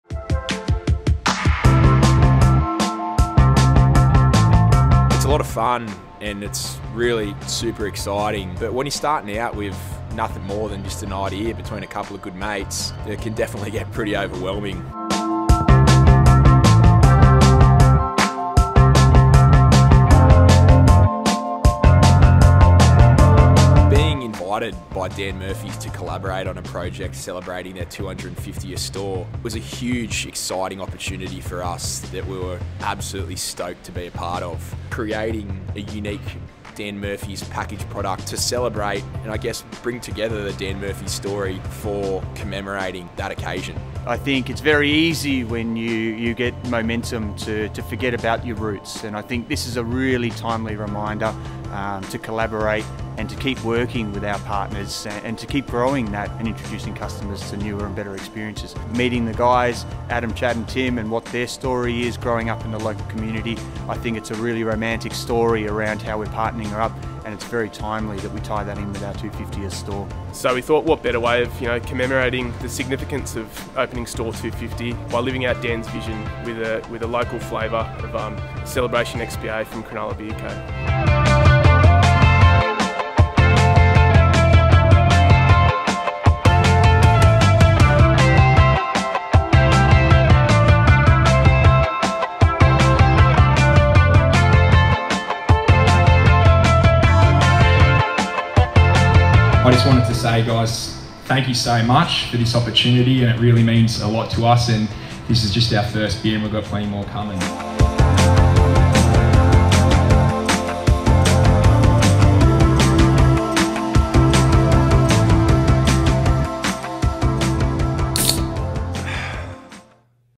DAN MURPHY'S x CRONULLA BEER CO. 250TH STORE COLLABORATION
We have worked with the number one brand in liquor in Australia to commemorate their 250th store nationally.

A huge congratulations to the whole Dan Murphy's team.